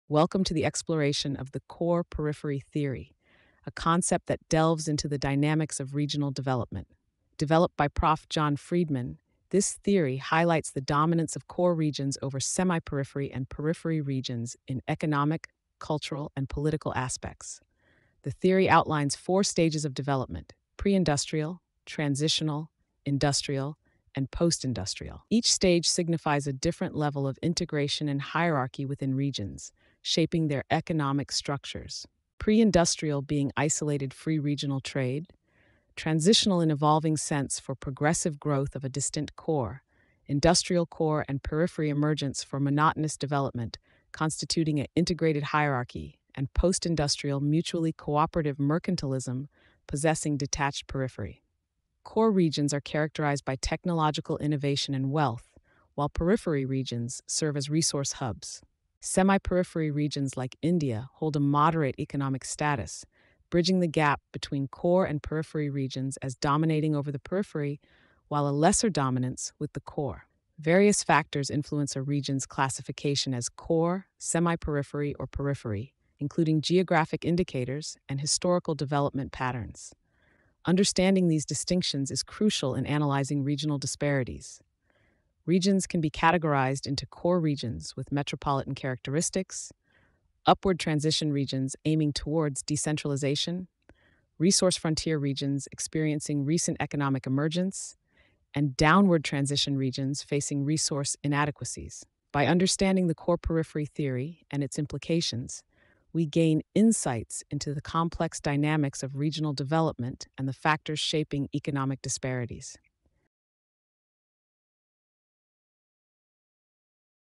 Understanding Core Periphery Theory in Regional Development
Welcome to the exploration of the Core-Periphery theory, a concept that delves into the dynamics of regional development.
 Developed by Prof John Friedmann, this theory highlights the dominance of Core regions over Semi-Periphery and Periphery regions in economic, cultural, and political aspects.
 The theory outlines four stages of development: Pre-Industrial, Transitional, Industrial, and Post-Industrial.
 Each stage signifies a different level of integration and hierarchy within regions, shaping their economic structures. ' Pre-Industrial' being isolated free regional trade,' Transitional' in evolving sense for progressive growth of a distinct core,' Industrial' core and Periphery emergence for monotonous development constituting a integrated hierarchy and' Post Industrial' mutually cooperative mercantilism possessing detached Periphery.
 Core regions are characterized by technological innovation and wealth, while Periphery regions serve as resource hubs.
 Semi-Periphery regions like India hold a moderate economic status, bridging the gap between Core and Periphery regions as dominating over the Periphery while a lesser dominance with the core.
 Various factors influence a region's classification as Core, Semi-Periphery, or Periphery, including geographic indicators and historical development patterns.
 Understanding these distinctions is crucial in analyzing regional disparities.
 Regions can be categorized into Core regions with metropolitan characteristics, Upward Transition regions aiming towards decentralization,
 Resource Frontier regions experiencing recent economic emergence, and Downward Transition regions facing resource inadequacies.
 By understanding the Core-Periphery theory and its implications,
 we gain insights into the complex dynamics of regional development and the factors shaping economic disparities.